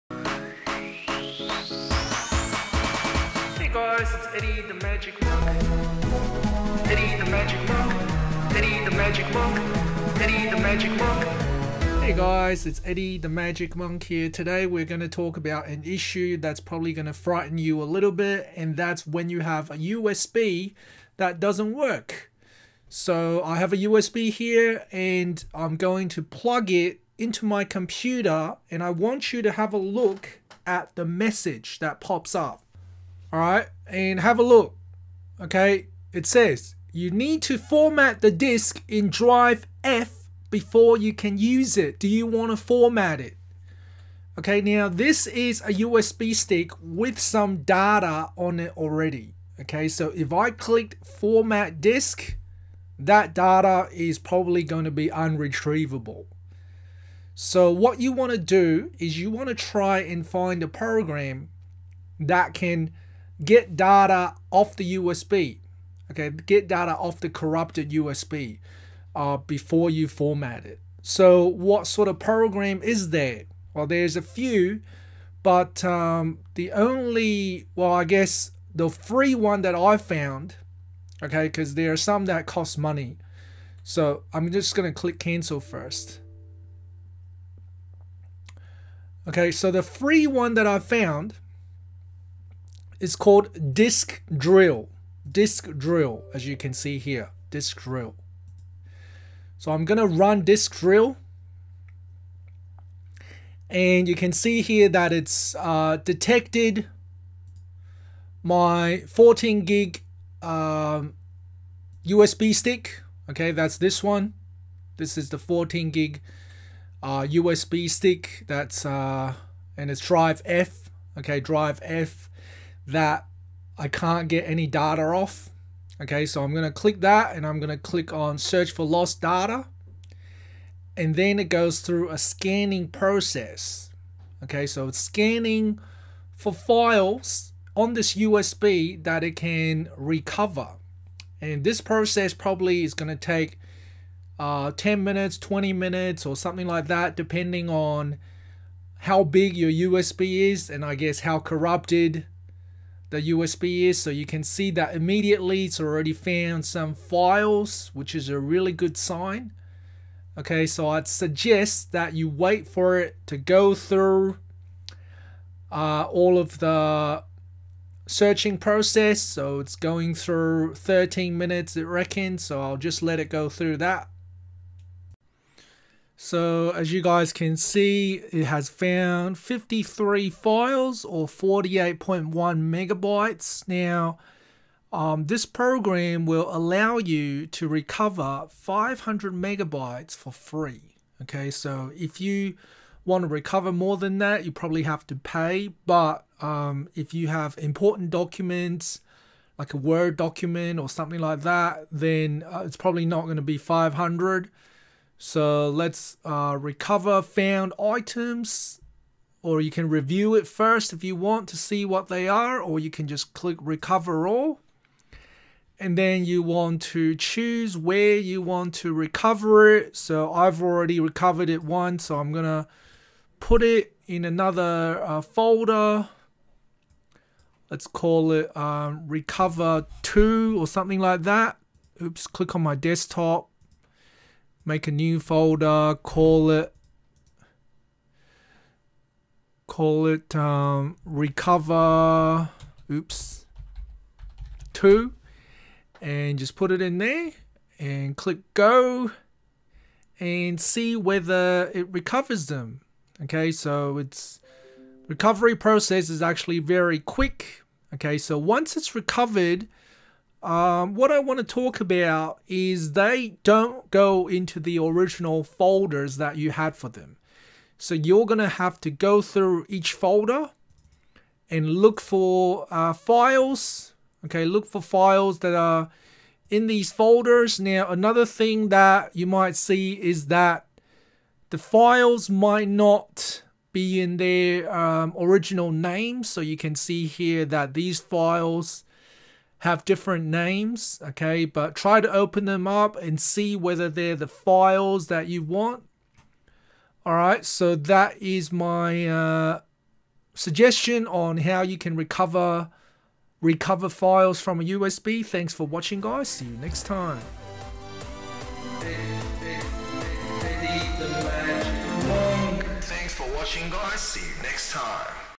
How to recover files from USB "You need to format the disk ..." (Corrupted)
When your USB becomes corrupted (moisture, age, repeitive / improper use etc) it might not even load at all. In this case where it says you need to format the disk, you can't even see what files are on there anymore. I show you an app which I downloaded to recover some important files.